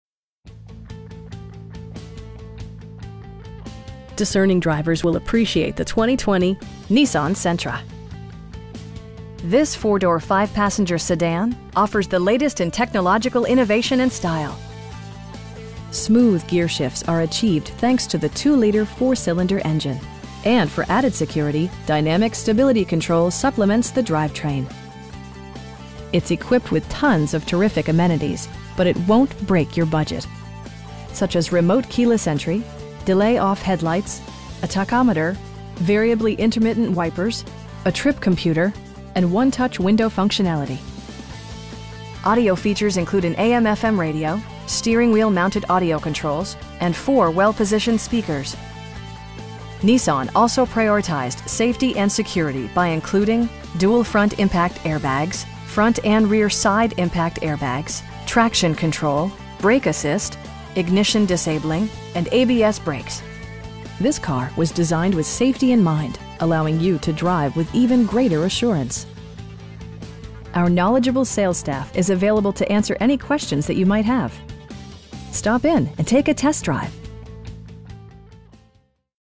2020 Nissan Sentra S in Fort Worth, TX 76116
3451 West Loop 820 S.

Discerning drivers will appreciate the 2020 Nissan Sentra. This 4 door, 5 passenger sedan offers the latest in technological innovation and style. Smooth gearshifts are achieved thanks to the 2 liter 4 cylinder engine, and for added security, dynamic Stability Control supplements the drivetrain. It's equipped with tons of terrific amenities, but it won't break your budget. Such as remote keyless entry, delay-off headlights, a tachometer, variably intermittent wipers, a trip computer, air conditioning, an overhead console, and 1-touch window functionality. Audio features include an AM/FM radio, steering wheel mounted audio controls, and 4 speakers, providing excellent sound throughout the cabin. Nissan also prioritized safety and security by including: dual front impact airbags, front and rear side impact airbags, traction control, brake assist, ignition disabling, and ABS brakes. This car was designed with safety in mind, allowing you to drive with even greater assurance. Our knowledgeable sales staff is available to answer any questions that you might have. They'll work with you to find the right vehicle at a price you can afford. Stop in and take a test drive!